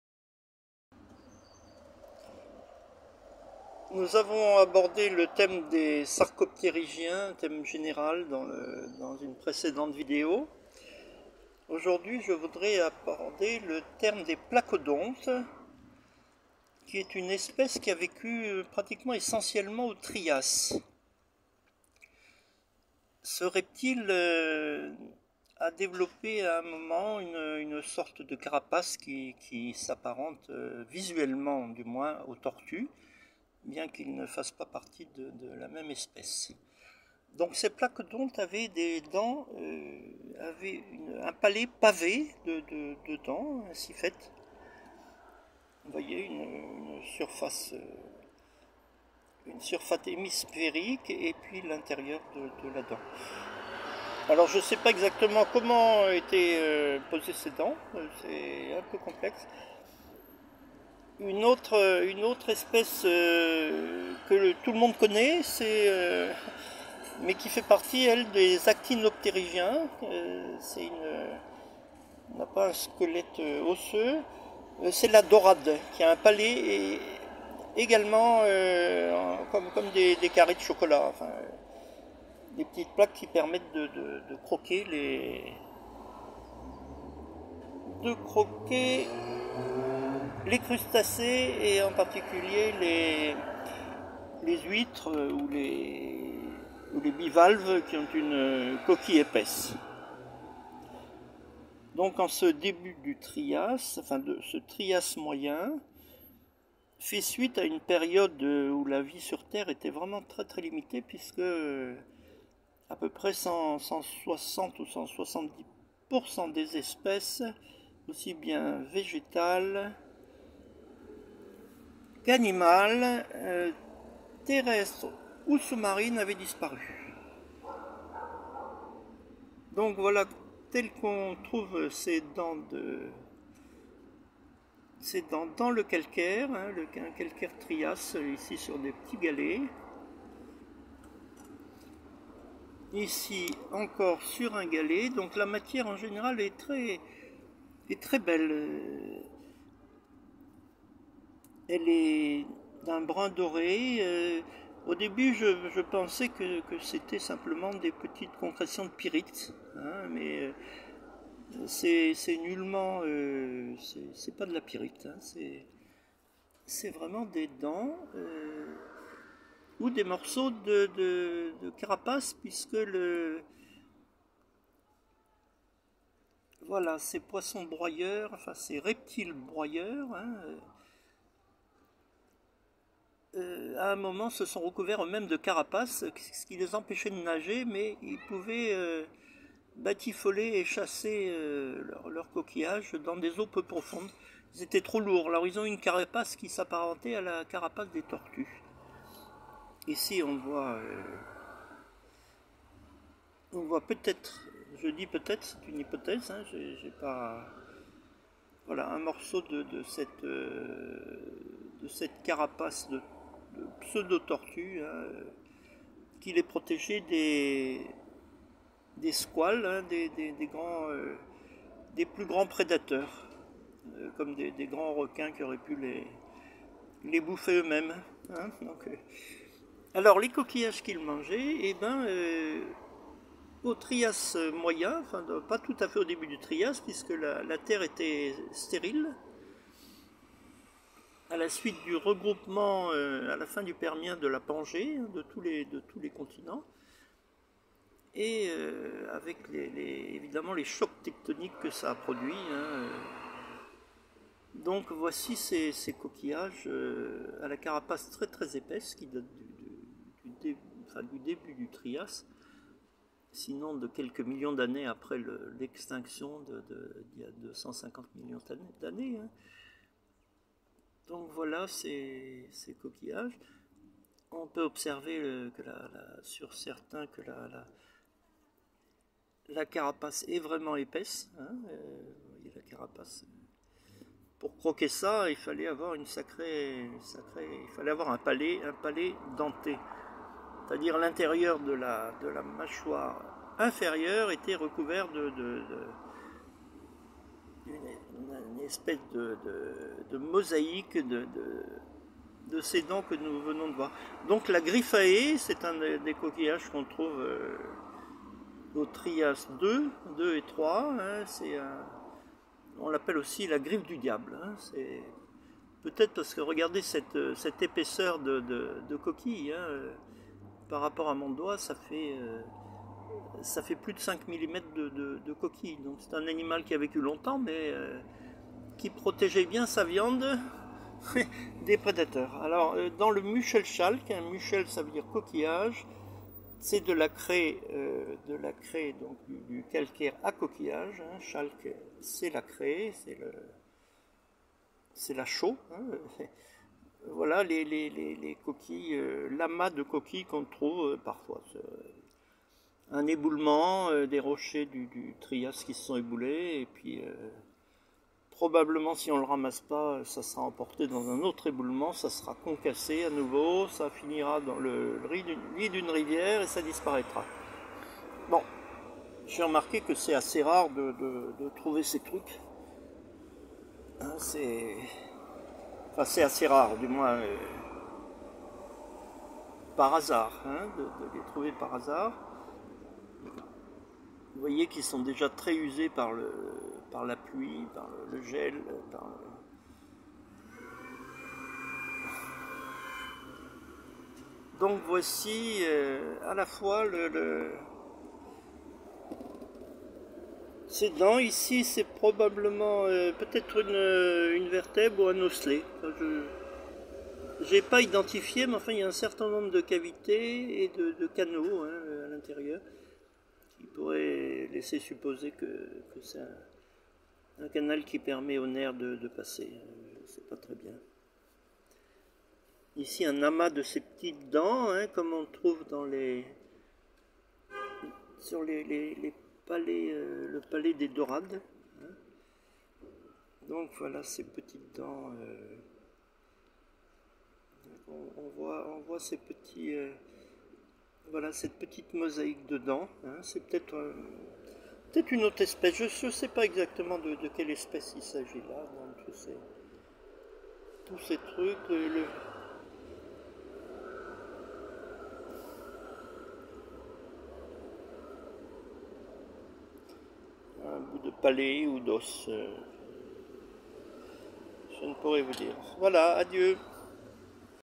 sauropterygii :le Placodonte du Trias et ses proies,les gryphaes(à Jean Vermeulun,paleontologue (+)
apres la grande extinction permo -trias ,la vie rendue impossible sur terre à cause des chocs tectoniques dus au rassemblement des continent dans la Pangée,la diminution du littoral,l'ecoulement de lave du trapp de Siberie sur des millions d'hectares ,la fin de l'erection de la chaine hercynienne ,le volcanisme Permien...la vie lentement reprend .les reptiles mammaliens ont disparu  ,+de 60% des animaux,des vegetaux,des creatures terrestres ou marines.
le Trias va voir apparaitre les dinosaures et d'autres especes.
certaines qui ont survecu vont evoluer dans cette niche ecologique tres reduite :les reptiles tetrapodes (Sauroptérigiens)
,dont le Placodonte dont je montre ici quelques dents,au palais recouvert d'un pavement de dents qui pourra ainsi broyer les bivalves à coquille dure pour manger ces coquillages ,comme le casse noix qui nous permet de manger la noix.
(remarque:la dorade a aussi une machoire broyeuse palatale ,mais fait partie des Actinopterigiens (Sparidés),ainsi que les Labridés qui ont aussi une "gueule pavée).